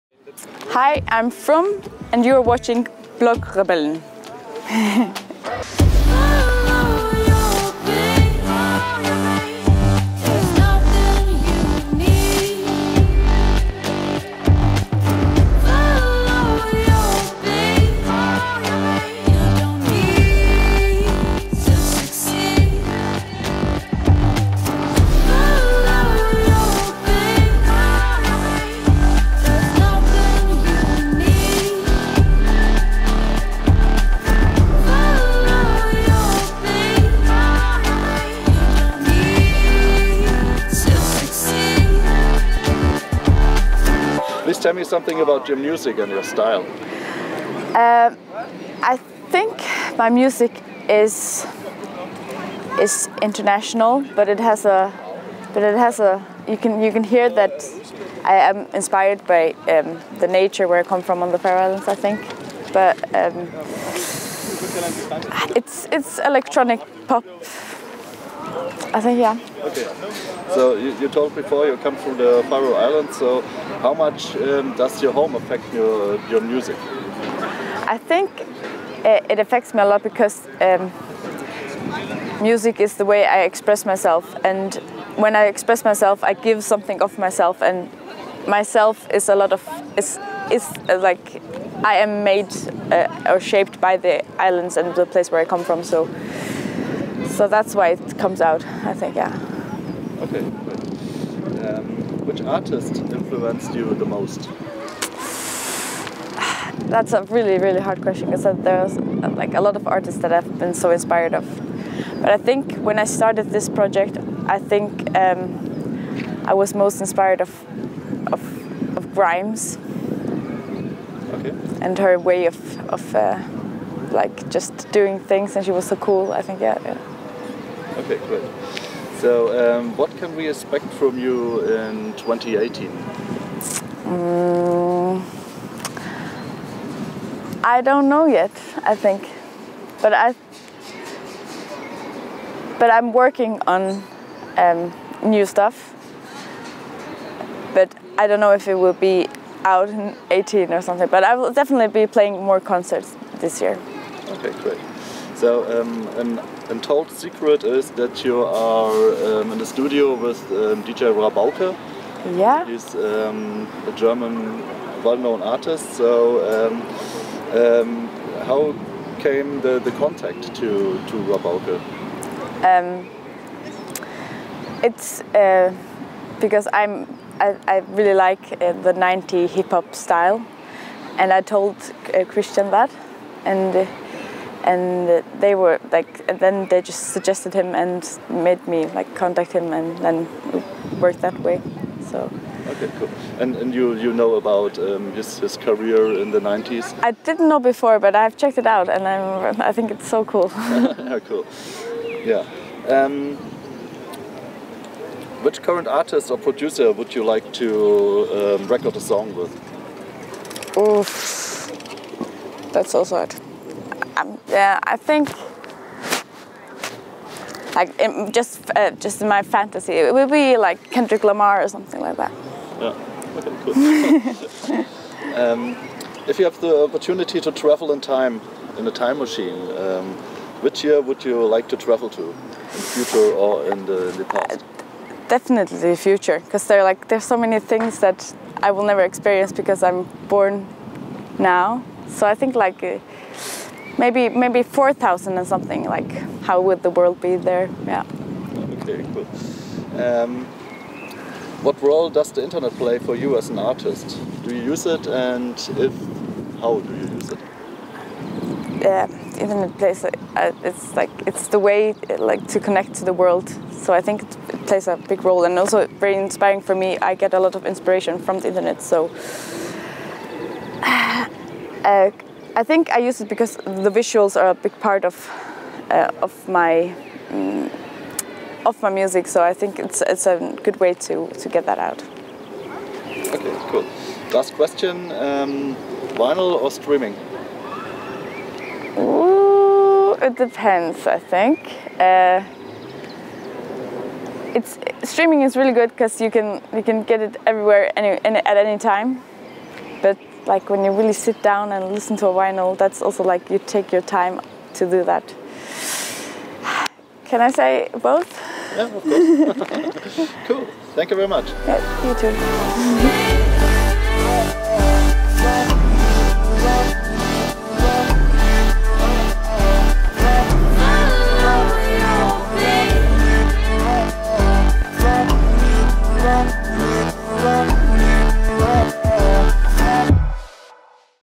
Interview with FRUM: Is she producing with DJ Rabauke?
It was the second time I met singer FRUM from Faroe Islands. The first time was in March when I visited the Faroe Music Awards on the beautiful islands and than I met her at SPOT Festival in Aarhus, Denmark, where I was invited a few weeks ago.

Jenny, that's FRUMs real name, was performing at this "showcast festival" and again I was impressed of her show because she swapped her dancers for a band and played some new songs.

Luckily there was a little space for a small talk with her about her music and a very special rumor: Is DJ Rabauke, a german artist known from EinsZwo, really producing some new music with her?